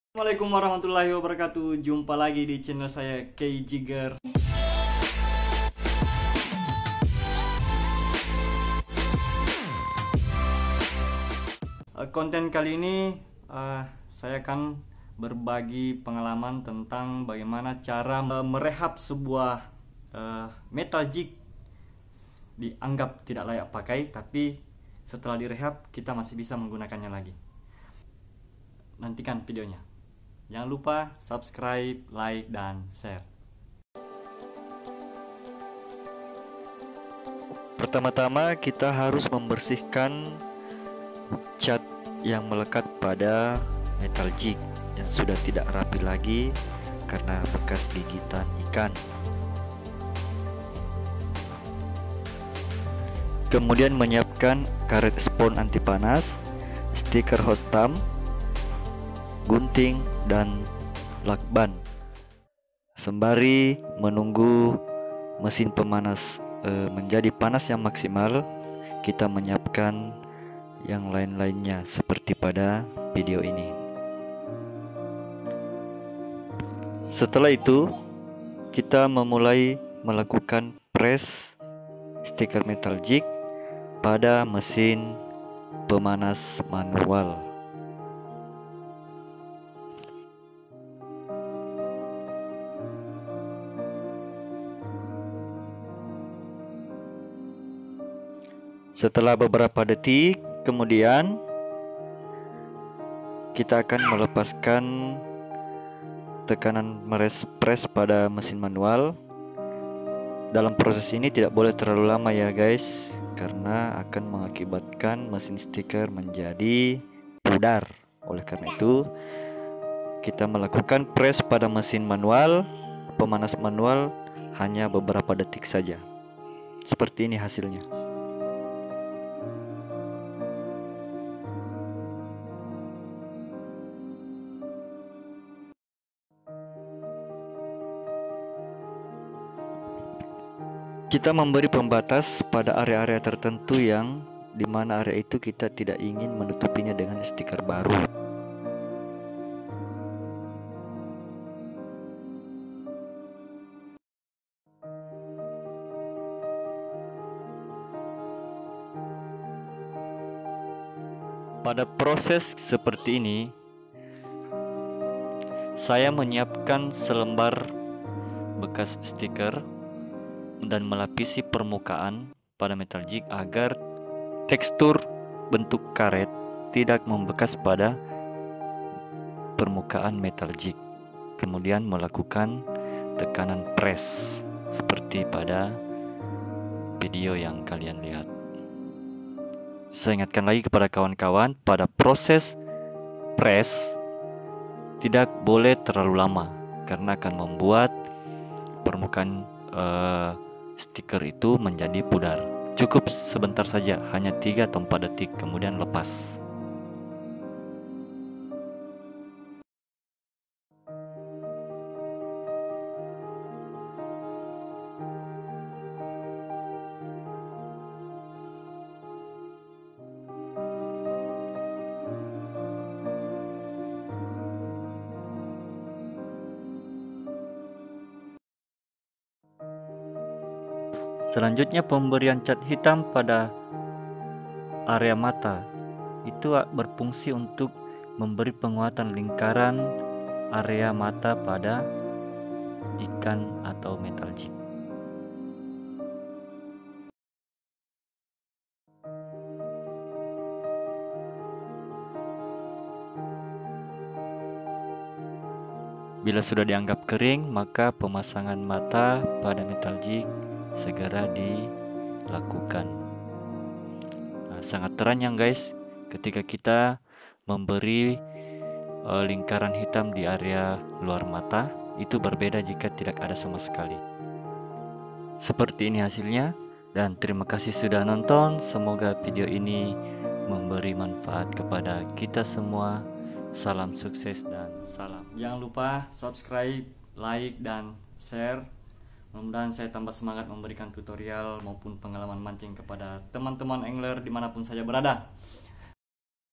Cara Pasang Transfer Foil Hologram Pada metal jig
Video kali ini menguraikan secara singkat tentang cara pasang transfer foil hologram atau stiker hotstamp metal jig dengan bahan dan alat sederhana agar metal jig menjadi baru kembali dan dapat dipergunakan lagi.